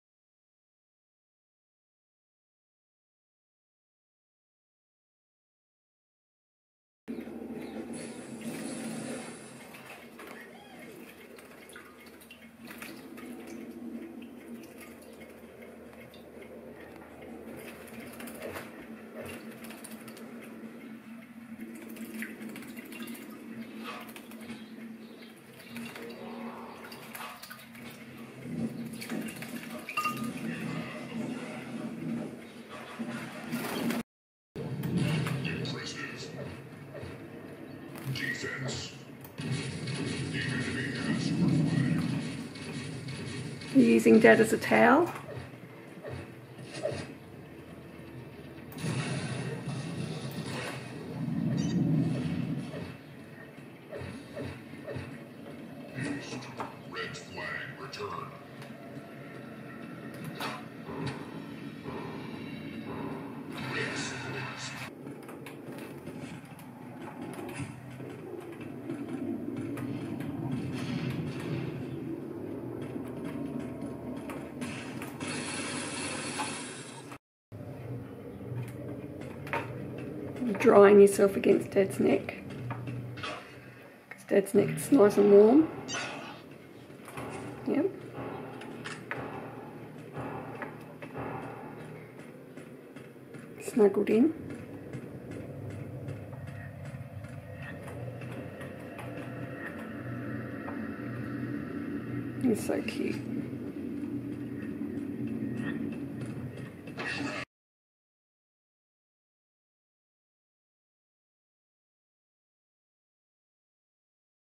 NORBU our cute pet sparrow has a bath and then uses us as a towel to dry off
If my husband is playing a pc game, Norbu will have a bath in the sink and then fly to him to dry off using his Dad's neck and warmth.